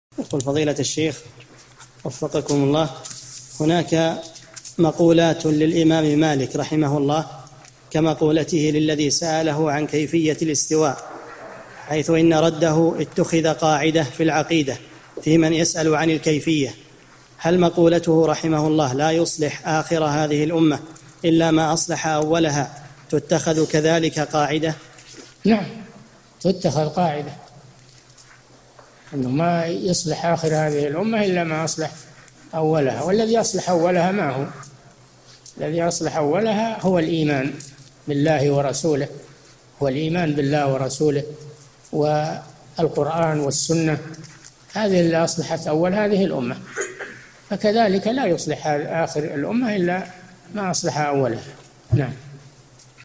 [602 /1350] قول الإمام مالك "لا يصلح آخر هذه الأمة إلا ما أصلح أولها" هل نتخذها قاعدة؟ صالح الفوزان
-[602 /1350] السؤال: هناك مقولات للإمام مالكٍ رحمه الله كمقولته للذي سأله عن كيفية الاستواء، حيث إن رده اتخذ قاعدةً في العقيدة فيمن يسأل عن الكيفية، هل مقولته رحمه الله: " لا يصلح آخر هذه الأمة إلا ما أصلح أولها "، تتخذ كذلك قاعدةً؟
 الجواب: نعم تُتخذ قاعدة، أنه ما يصلح آخر هذه الأُمة إلا ما أصلح أولها، والذي أصلح أولها ما هو؟ الذي أصلح أولها هو الإيمان باللهِ ورسوله، والإيمان بالله ورسوله والقُرآن والسنة هذا الذي أصلح أول هذه الأُمة، فكذلكَ لا يُصلح آخر الأمة إلا ما أصلحَ أولها.
للاشتراك في قناة الشيخ صالح الفوزان
أسئلة وأجوبة من شرح قاعدة جليلة ( 1350 ) مقطع

للاستفسار والإقتراحات والدعم
رئيس الفريق
لمشروع كبار العلماء